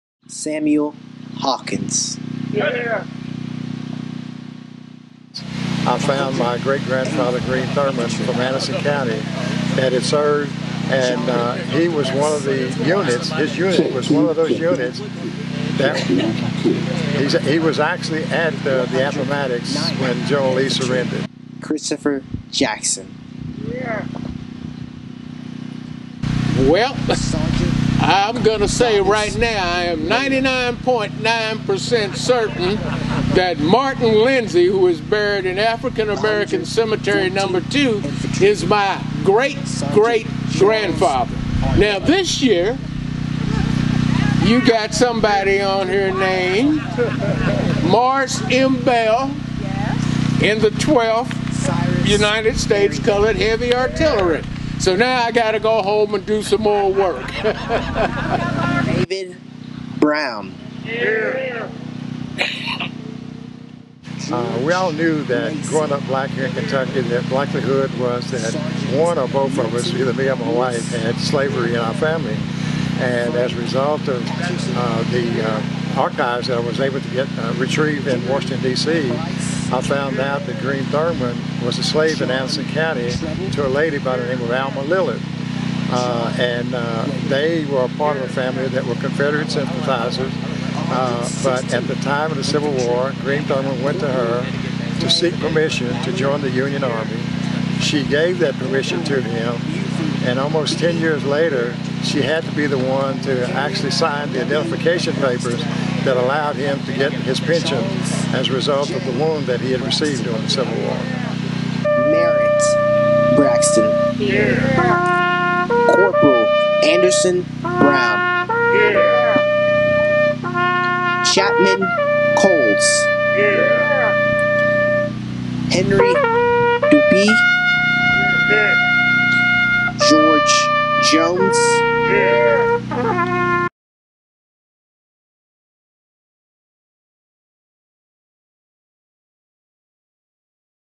African-American veterans of the Civil War honored at Juneteenth Jubilee
Black soldiers who served in the Civil War were remembered at Juneteenth Jubilee at Lexington’s African Cemetery No. 2 Saturday night. The names of 112 Civil War soldiers who are buried in the cemetery were read aloud.

Video by Matt Goins/Lexington Herald-Leader